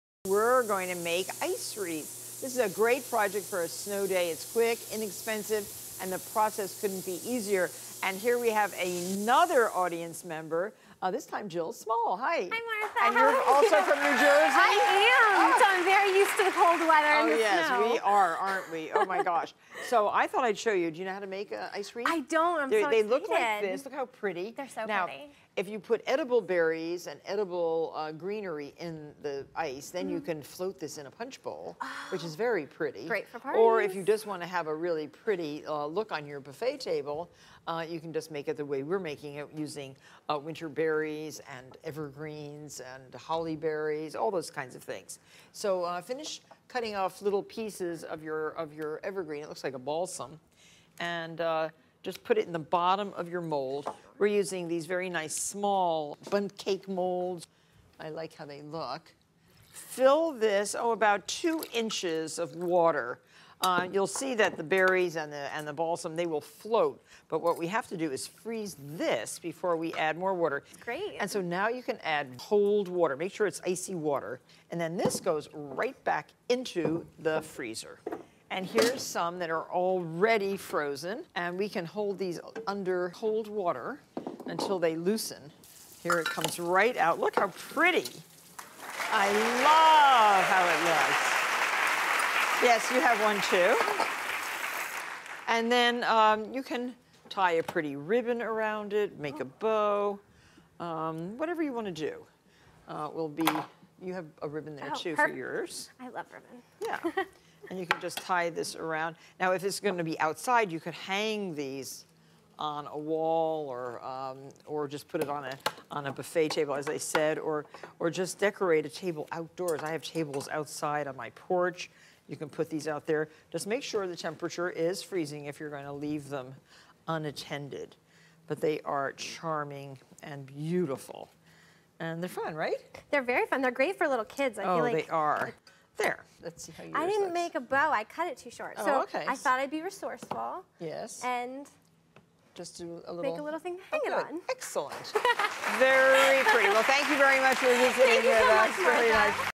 Making Ice Bundt Sculptures with Martha Stewart
Create Ice Bundts for holiday decorations by using Nordic Ware's 6-cup Bundt Anniversary Pan, which is Made in the USA.